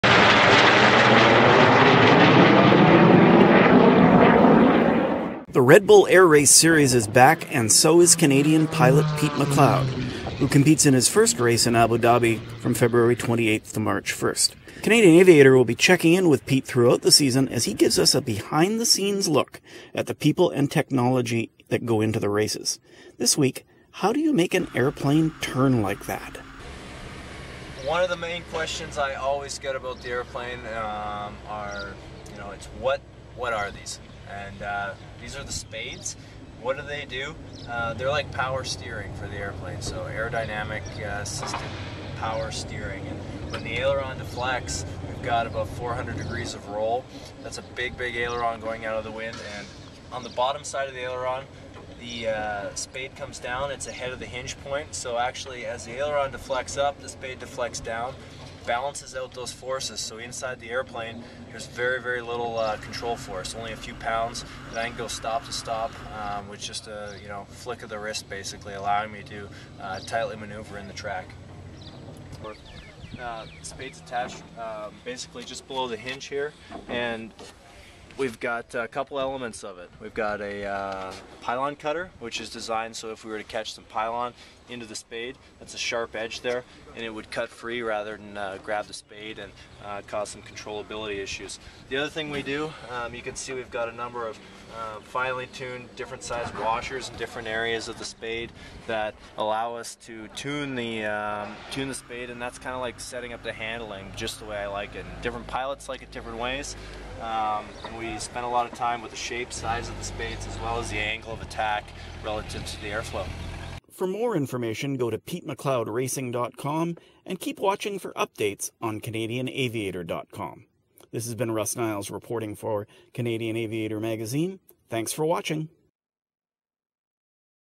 Pete McLeod Abu Dhabi
Red Bull racer Pete McLeod is getting ready for the series' first race in three years in Abu Dhabi. Here he explains one part of "How do they do that?"